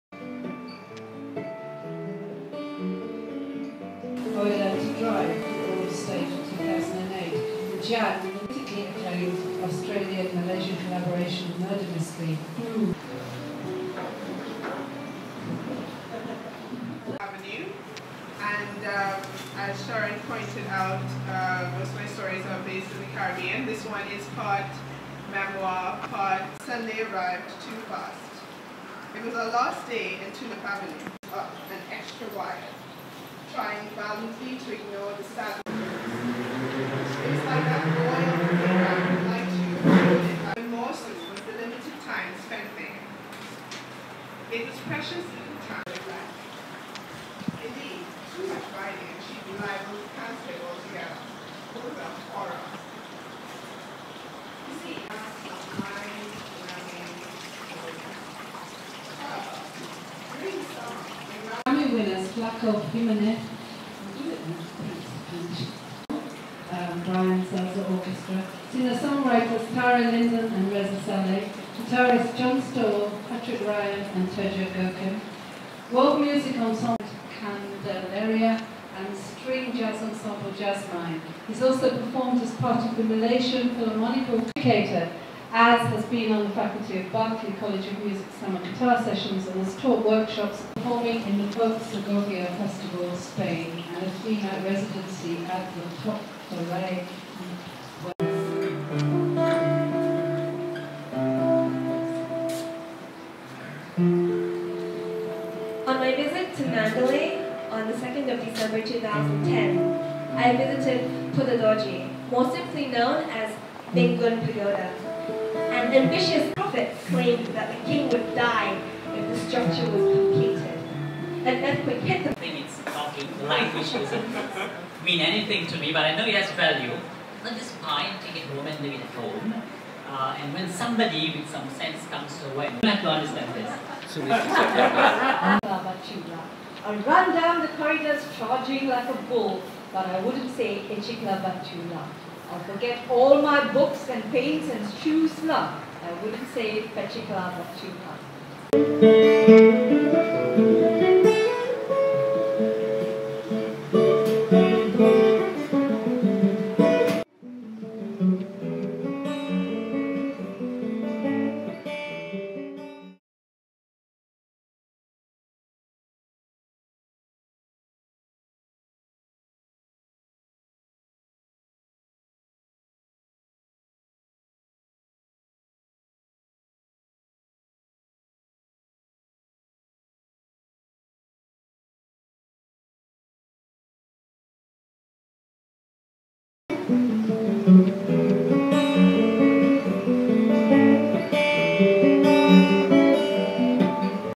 Readings Sept 2012 (names omitted)
Monthly readings in Seksan Gallery, Bangsar, Kuala Lumpur